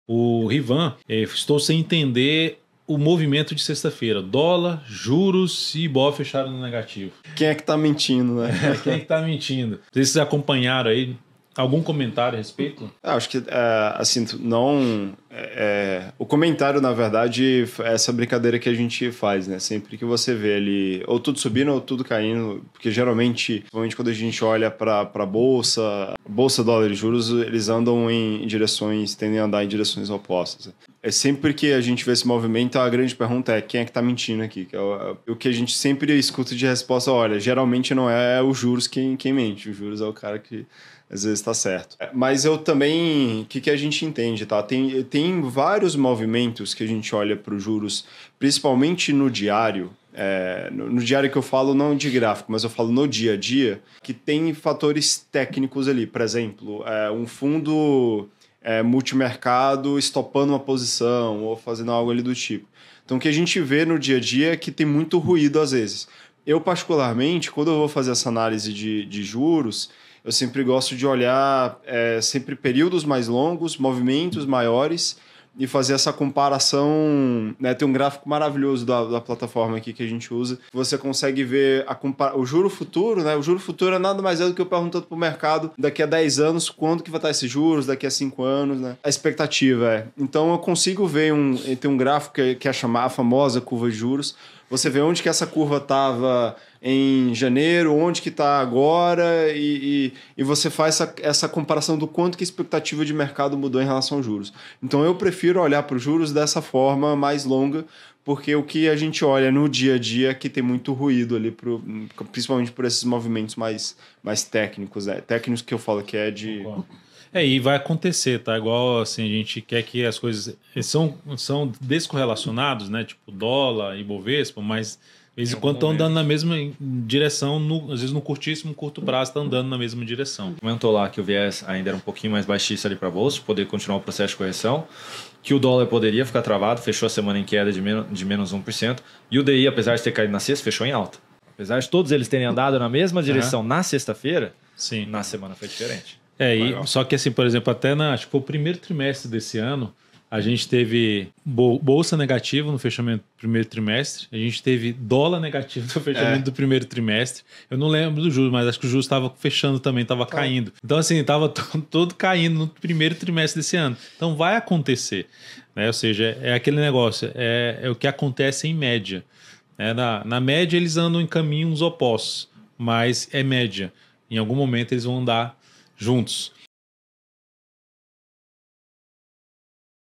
Quem está mentindo?
Dólar, juros e Ibov no negativo: tem alguém mentindo?
No pregão da última sexta-feira, 20 de outubro, aconteceu um movimento que chamou atenção dos investidores. Mas que movimento foi esse?
Porque será que pode ser que algum indicador esteja mentindo?

Neste corte da live especial DV 15 anos, o especialista Erik Sala comenta um pouco sobre a relação entre esses indicadores e revela se tem alguém mentindo ou não!

Acompanhe e deixe sua opinião nos comentários!